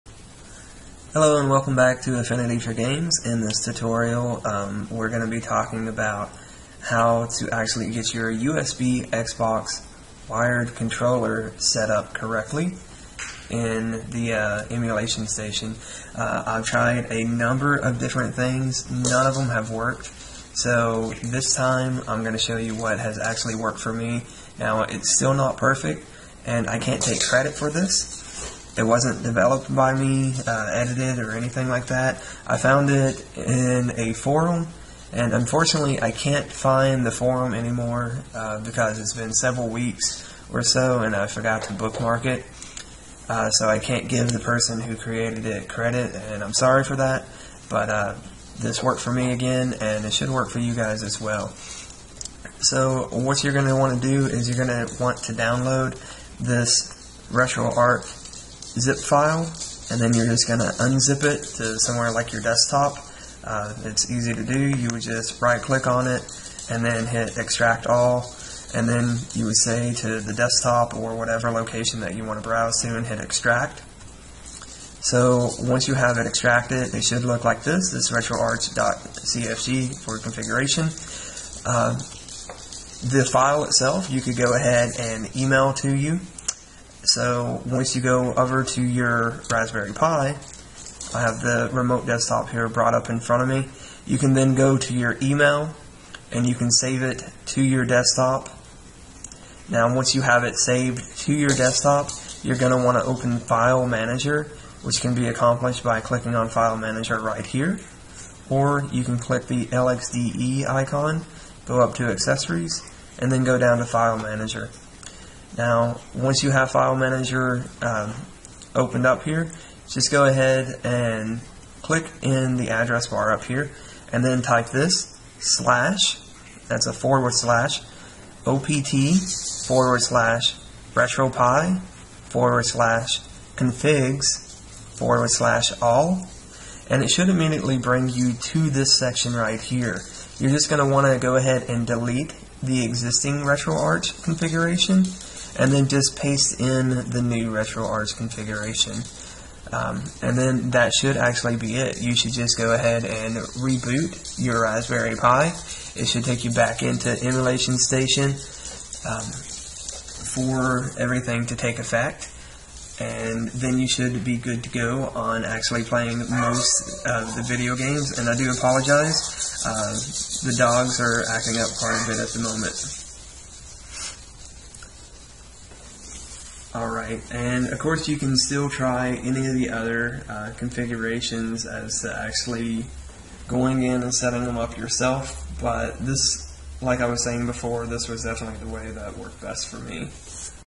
How to: Setup RetroPie v2.3 USB X Box 360 Controller
How to: setup the X Box 360 USB wired controller and other USB controllers for the RetroPie v2.3.

or